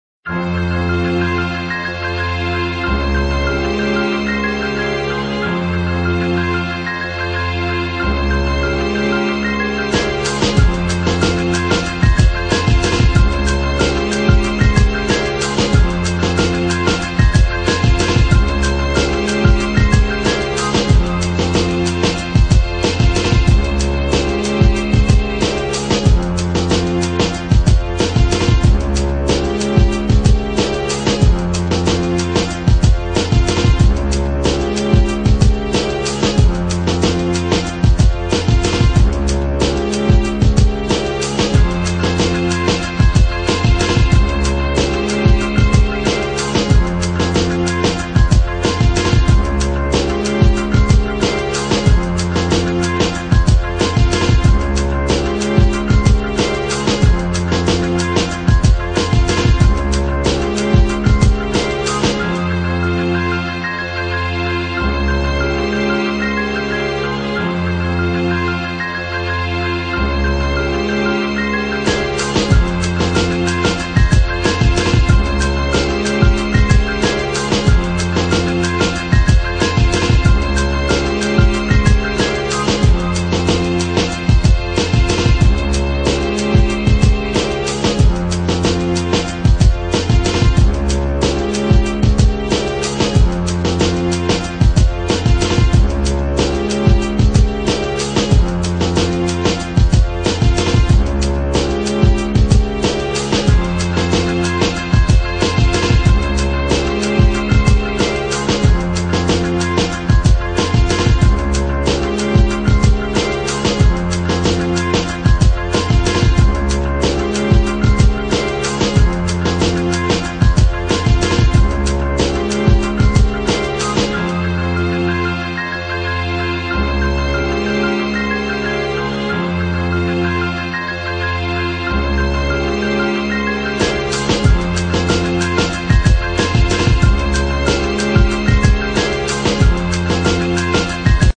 |[ Psychedelic Mammatus Clouds ]|
It was definitely different, trippy in away because there was a pinkish-red, yellow and orange illumination on the ground from the sky & clouds. Strange aura, yet beautiful!